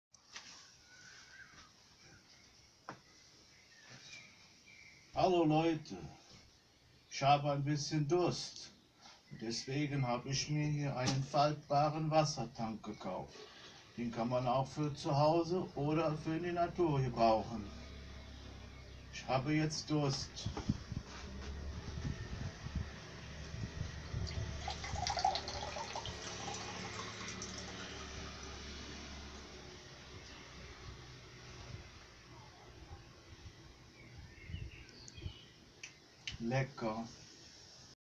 Testfootage "20 Liter Outdoor Wassertank-faltbar"
Mein erster Beitrag als Probe mit Eifelaner Dialekt... ;)

Hallo Leute,
ich habe aus Kanada mir einen faltbaren Wassertank besorgt.
Dieser eignet sich hervorragend in der Natur zum Überleben.
Er fasst 20 Liter und ist frei von Weichmachern.
Durch sein durchsichtiges Material kann der Tank in der Sonne stehen und die UV-Strahlen töten nach längeree Bestrahlung Bakterien und Keime.
An der Aufhängeöse, die diagonal zum Ablasshahn ist lässt sich der Tank auch als Dusche umfunktionieren.